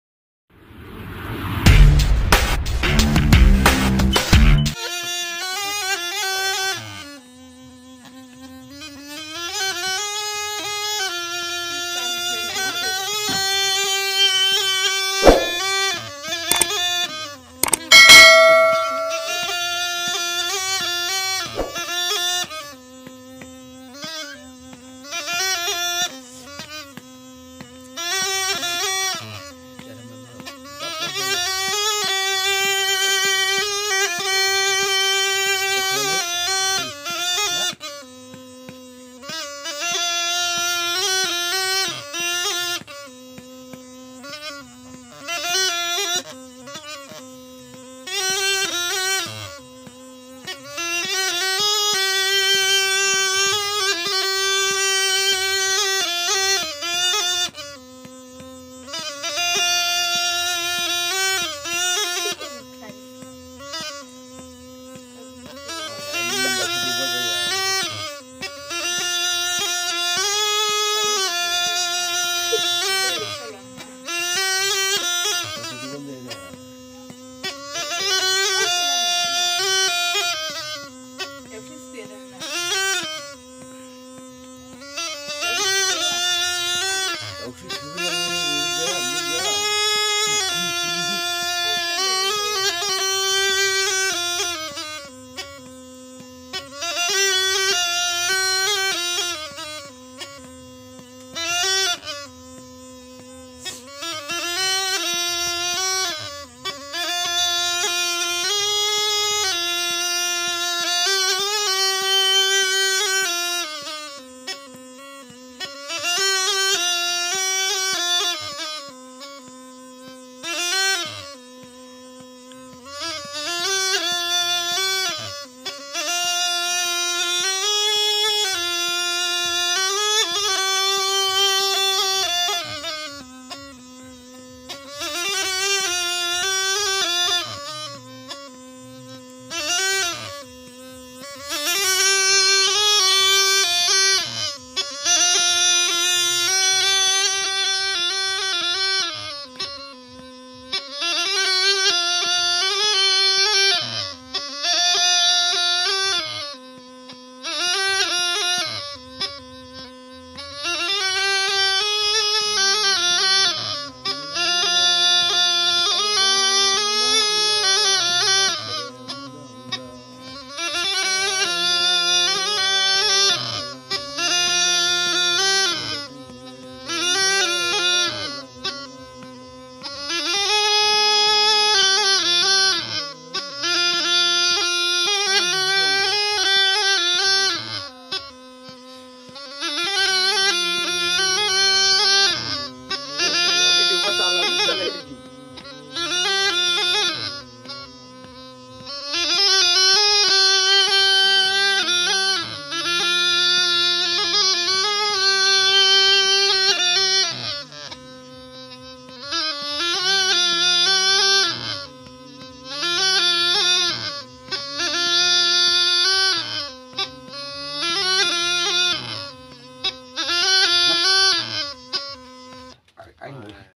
Mama playing dimasa traditional fluid ( watesa) # razut vlogs.
Mama playing dimasa traditional fluid
( Watesa) # razut vlogs.
1 .please watch my video and
3 .langthasarazut###.